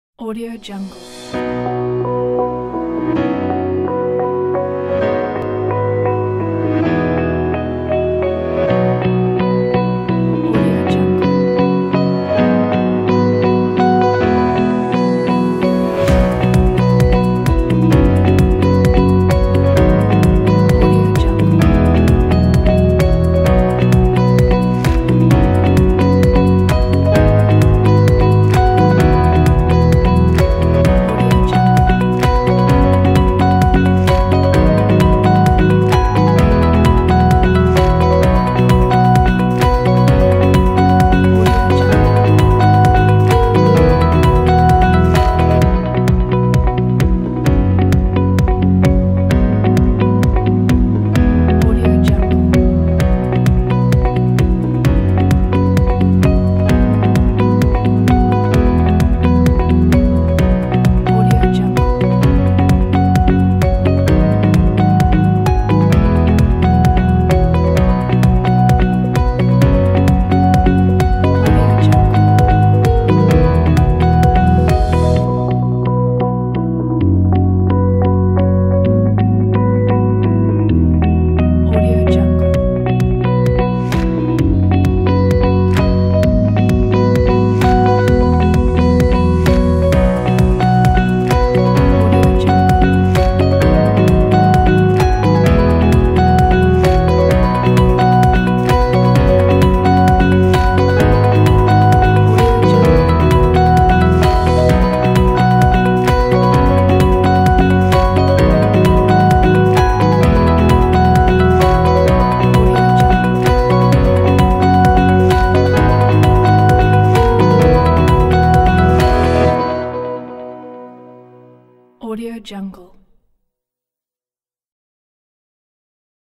Uplifting & Inspiring Background Music for Videos & Presentations
Inspiring fresh modern background music for travel, vlogs, advertising and other media projects.
If you want to use this track - you need a license.